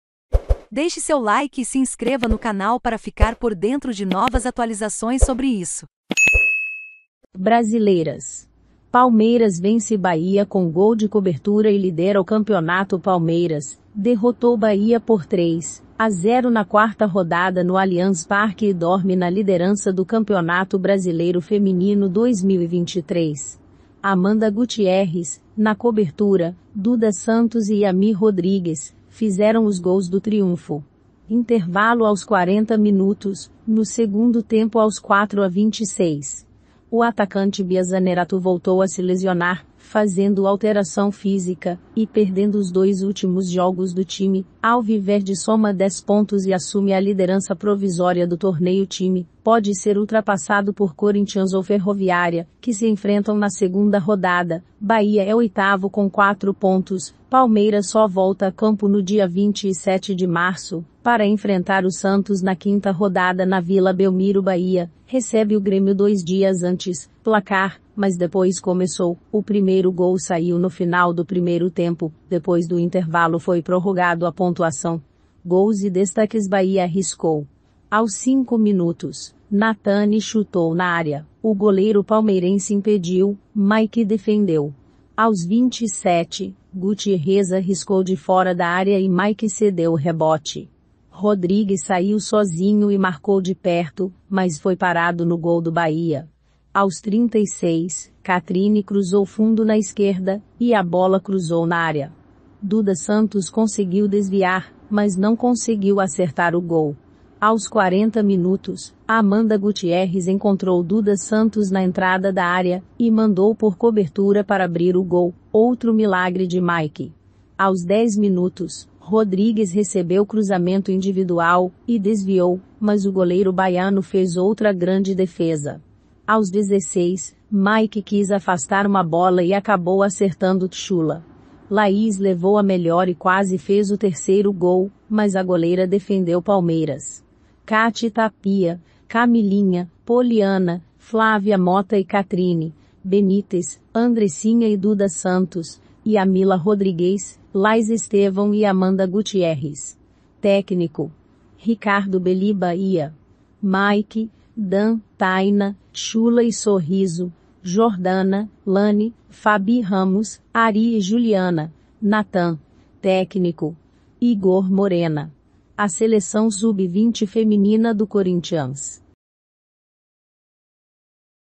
BRASILEIRAS: PALMEIRAS VENCE BAHIA COM GOL DE COBERTURA E LIDERA O CAMPEONATO
Palmeiras vence o Bahia por 3 a 0 na quarta rodada no Allianz Parque e cai no sono na liderança do Brasileiro Feminino 2023. Gols e DestaquesBahia corre perigo. Aos 5 minutos de jogo. Natane chutou na área e o Palmeiras goleira impedida das. Palmeiras: Kate Tapia; Camilinha. Poliana. Flávia Mota e Katrine; Benítez. Andressinha e Duda Santos; Yamila Rodríguez. Laís Estevam e Amanda Gutierres. Técnico: Ricardo Belli.